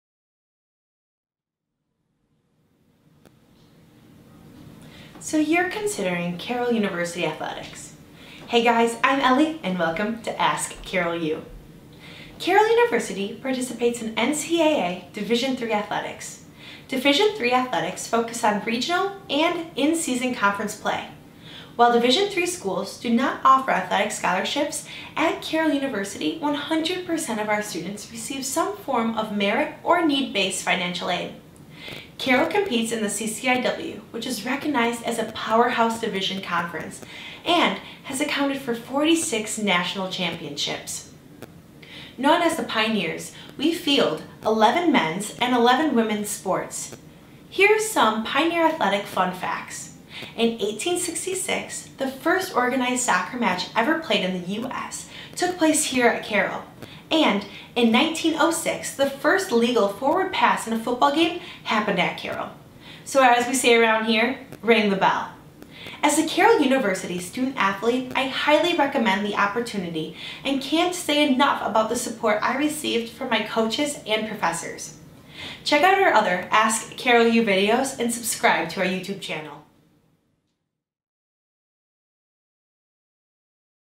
Athletics | Carroll University
Carroll University participates in NCAA, Division III athletics. Division III schools provide an integrated environment focusing on academic success while offering a competitive athletics environment.  Division III rules minimize potential potential conflicts between athletics and academics and focus on regional in-season and conference play.

While Division III schools do not offer athletic scholarships, at Carroll University, 100% of our students receive some form of merit or need-based financial aid. According to the NCAA, is 87% of the NCAA Division III athletes graduate from college.

Carroll University competes in the CCIW, which is recognized as a powerhouse Division III conference - and has accounted for 46 national championships!

Known as the "Pioneers", Carroll fields 11 men's and 11 women's sports.

Pioneer Athletic Fun Facts:
1. In 1866, the first organized soccer match ever played in the US took place her, at Carroll.
2. In 1906, the first legal forward pass in a football game happened at Carroll.